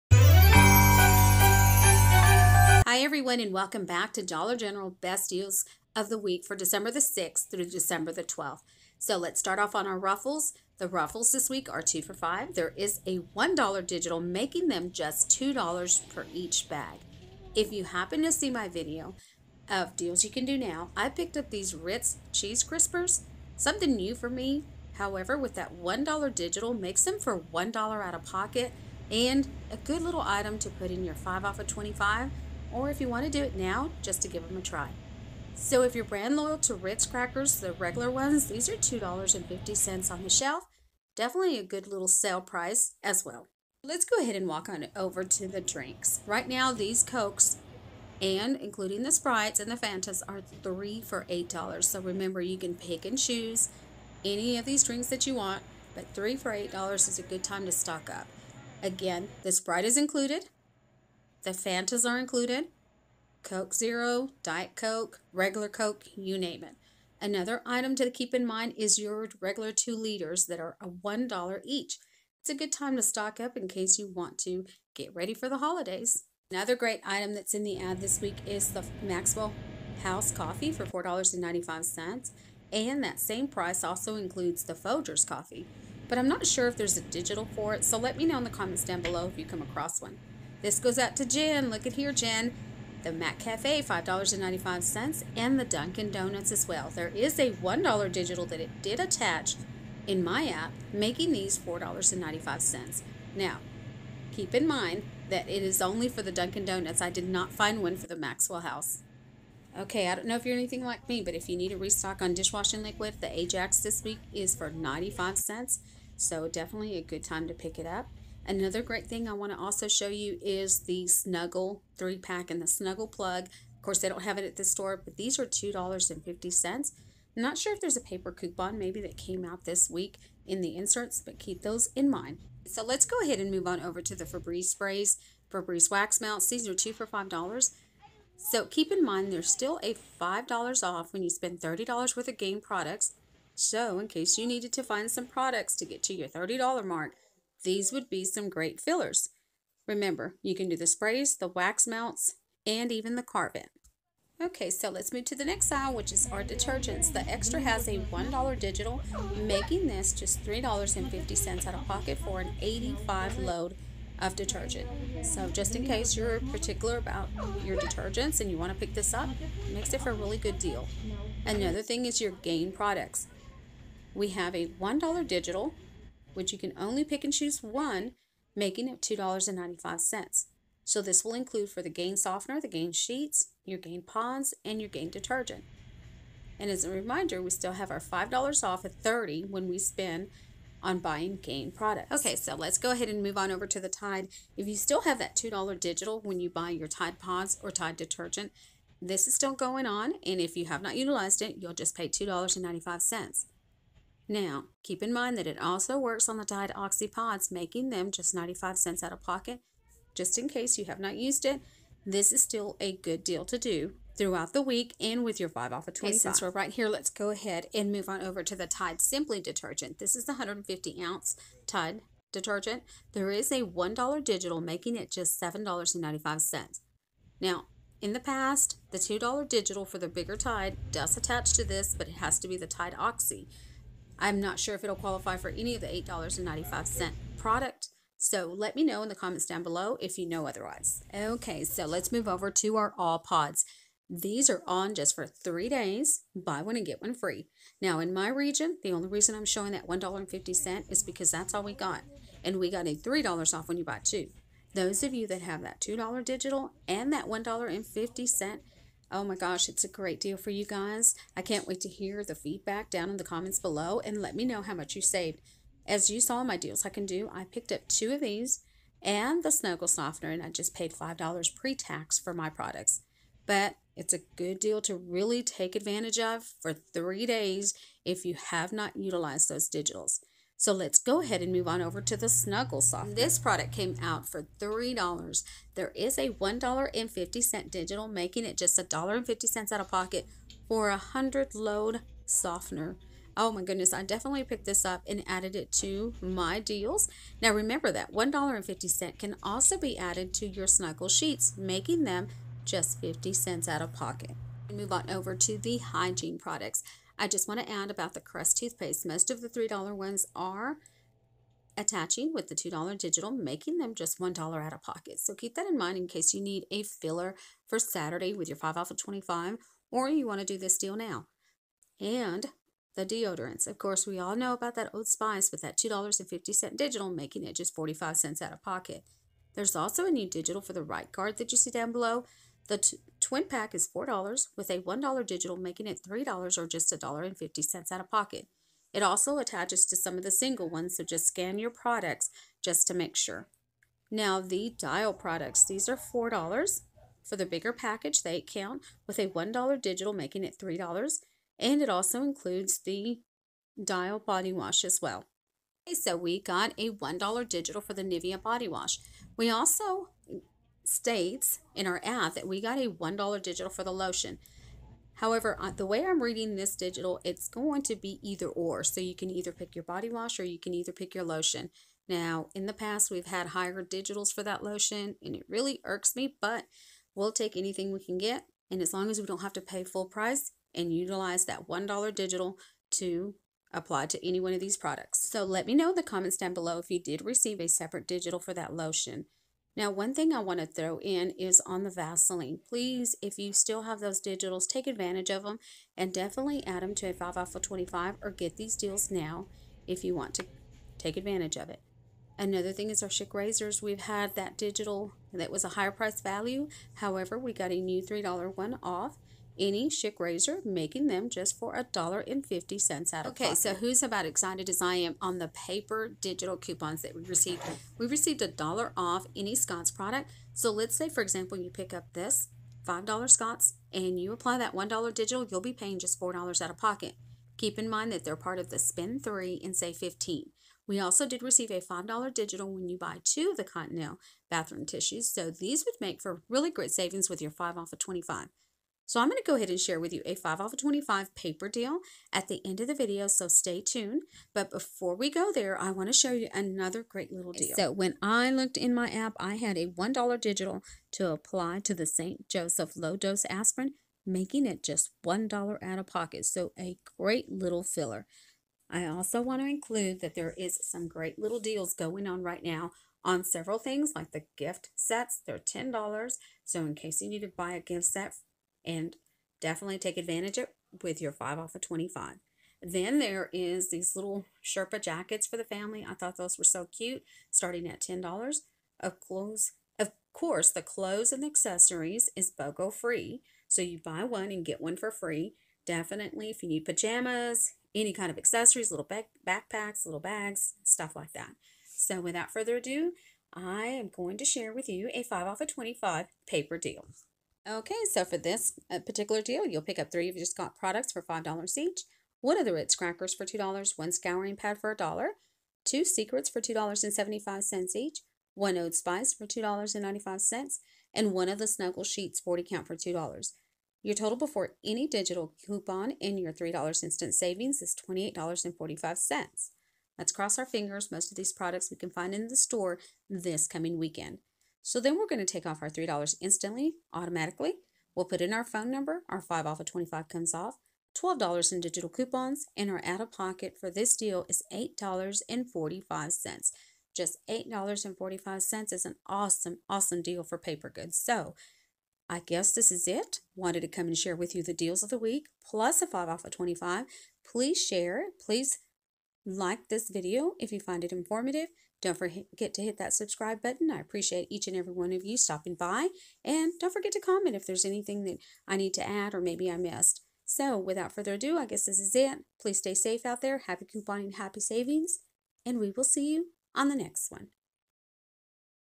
Dollar General deals of the week 12/6-12/12 Plus a $5 off $25 Scenario
Dollar General deals of the week12/6-12/12./Plus a $5off$25 paper scenario

Join Fetch Rewards and earn 2,000 points on your first purchase you upload

Inserts
Wecoupon clipz
Sunday paper